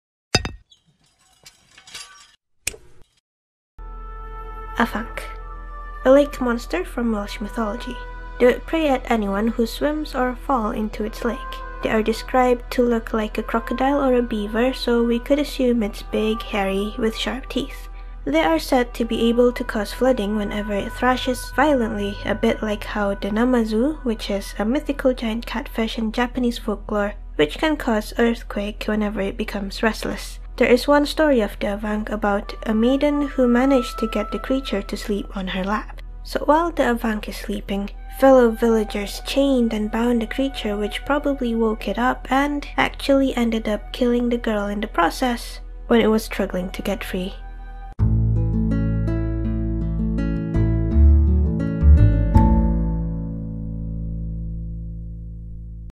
Afanc, the Sharp Teethed Fluffy Creature - Welsh Folklore
A lake monster from Welsh Mythology. They would prey at anyone who swims or fall into its lake. They are described to look like a crocodile or a beaver so we could assume its big, hairy with sharp teeth.